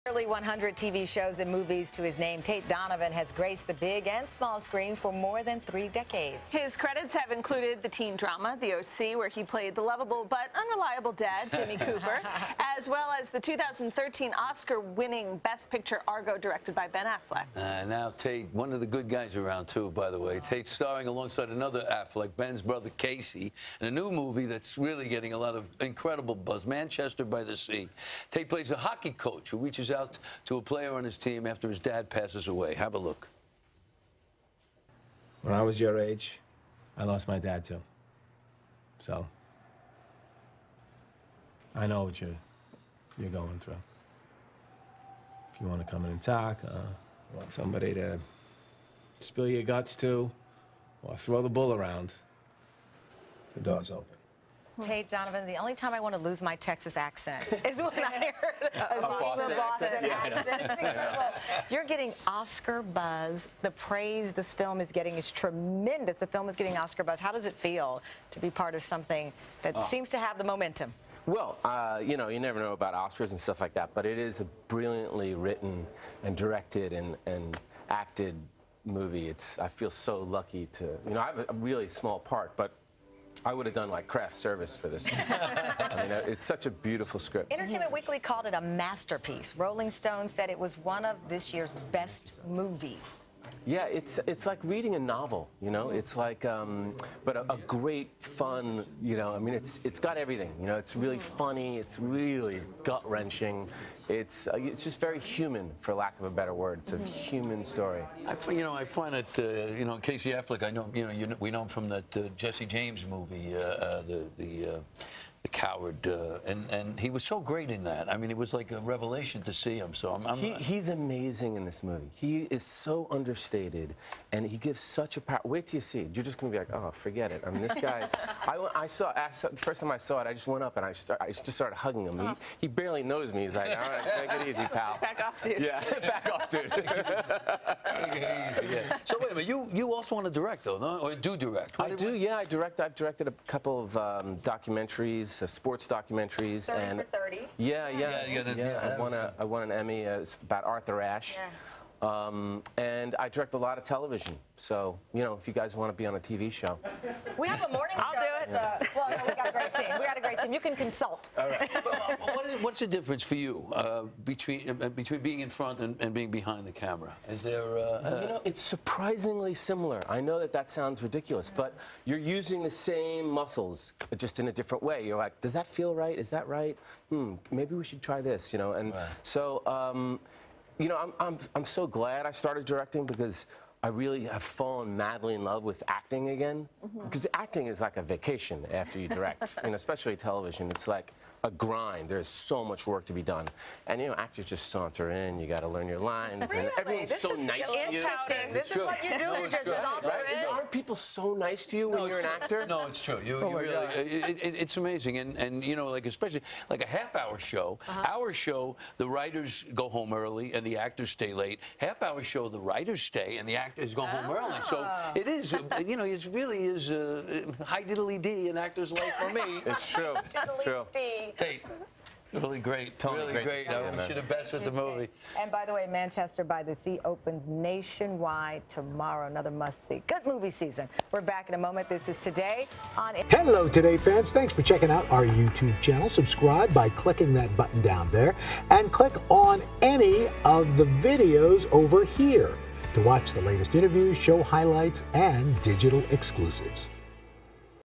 Tate Donovan: ‘Manchester By The Sea’ Is ‘Brilliant’ | TODAY
Known for his roles in such TV series as “The O.C.” and “24: Live Another Day,” Tate Donovan is returning to the big screen in the critically acclaimed drama “Manchester by the Sea,” which hits theaters nationwide Friday. The actor tells TODAY that the film is “brilliantly acted and directed.”